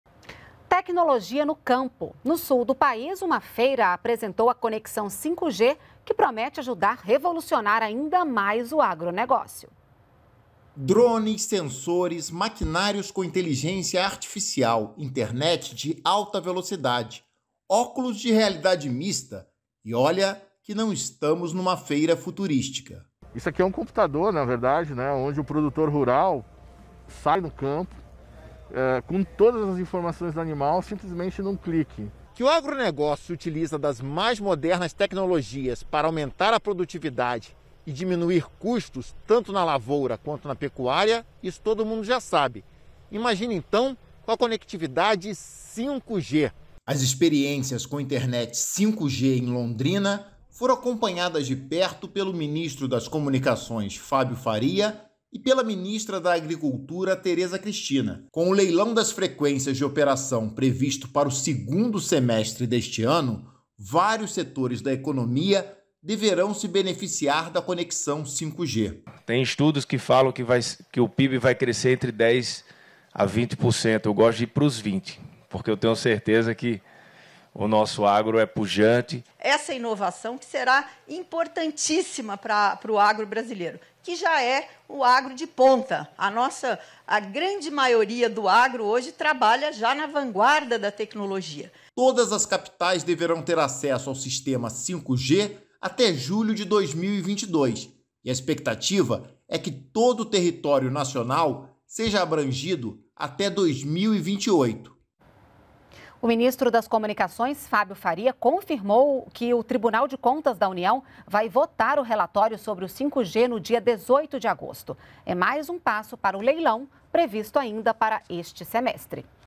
Tecnologia no campo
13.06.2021 – Inovação e internet rápida devem melhorar os negócios no campo. Em Londrina, no Paraná, uma feira apresentou a conexão 5G, que promete ajudar a revolucionar ainda mais o agronegócio.